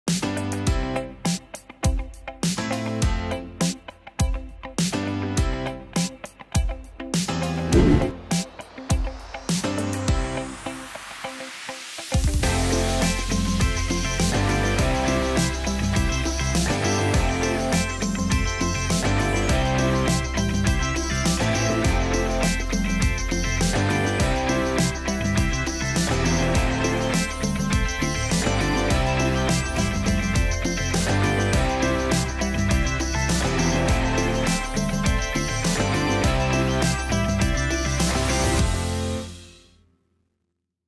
Nabel Sintered Stone and Bentley Automobile 4S Shop
Nabel Sintered Stone works a lot with Automobile 4S Shop. They are impressed by our stone look. It feels like real marble world. Amazing job from Nabel team.